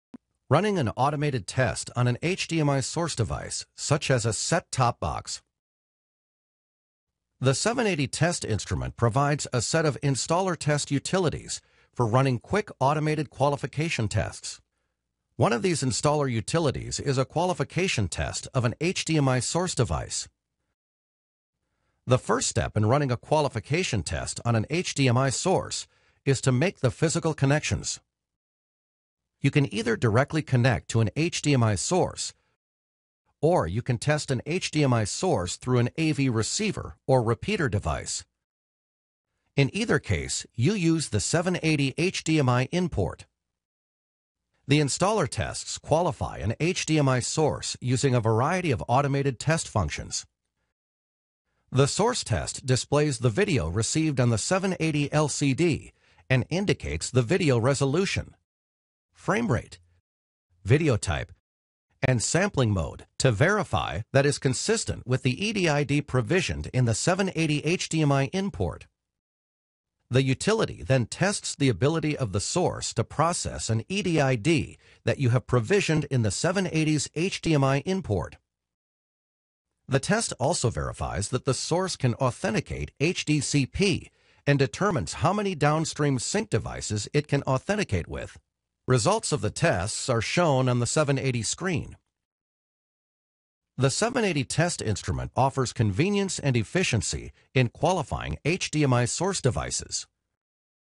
Testing HDMI Source Devices with the Quantum Data 780 Test Instrument
The 780 Handheld Test Instrument for HDMI® is a battery-powered, portable video and audio generator and HDMI analyzer that enables you to conduct quick, on-site verification testing and troubleshooting of your HDMI system and analog video displays. The 780 is equipped with both a reference HDMI source and a reference HDMI sink interface allowing you to test audio, video and HDMI protocols—HDCP, EDID, CEC & infoframes—of any type of HDMI device: sources, repeaters and sinks.